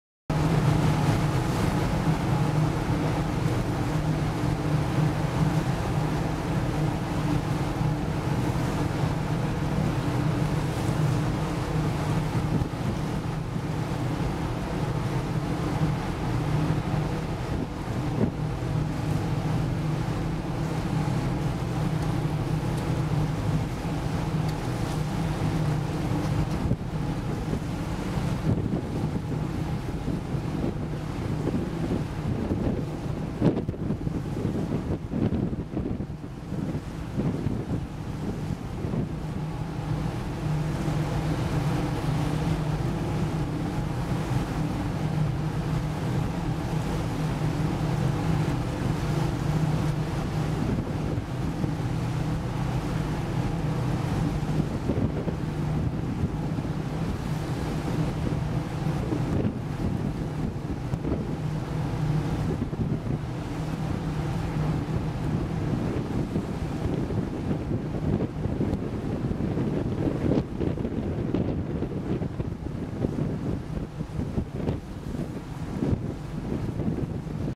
MS Nordlys - Beiboot für Gletscherausflug
Beiboot kommt um Personen für den Gletscherausflug aufzunehmen..